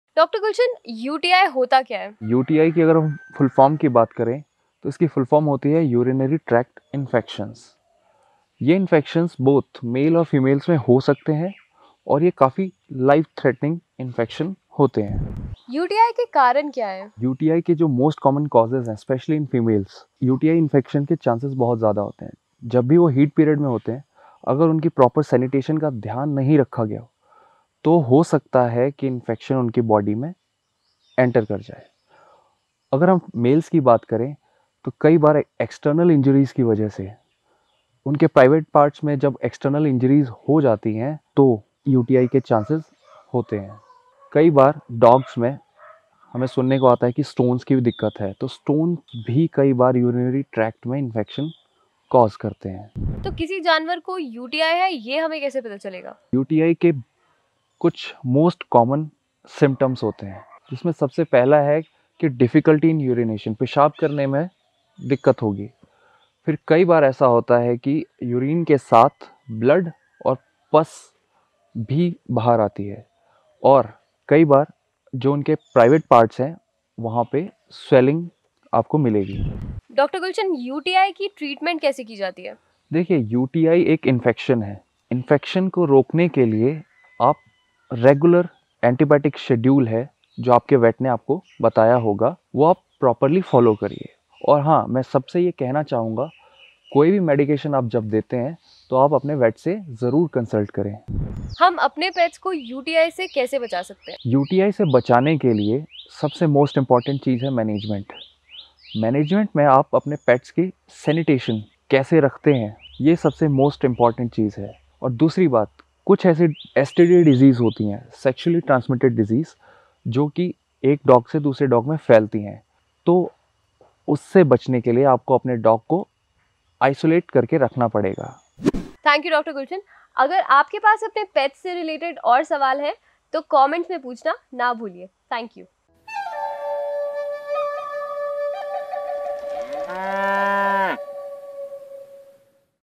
All about UTI | Urinary Tract Infections | UTI in dogs | Ask a vet!
Insaanon ki tarah, dogs ko bhi Urinary Tract Infections bhi ho sakte hain.

Iss video mein Dr. Gulshan bataate hain ki in infections ko kaise pehchaane, aur inse apne pets ko kaise bachaye.